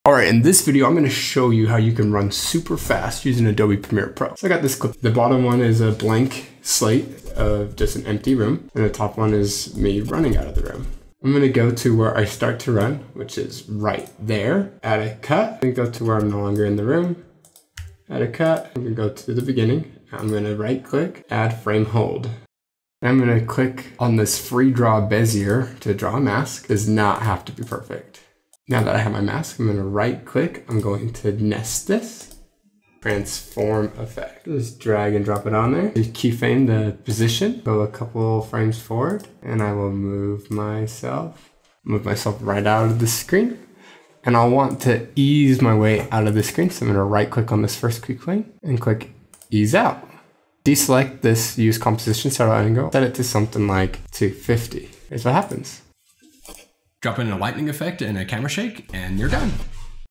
How to run with super speed effect in Adobe Premiere Pro
A tutorial on creating the superspeed effect in Premiere Pro in 1 minute.

It was $99 and had tons of superhero effects you just drop into your video.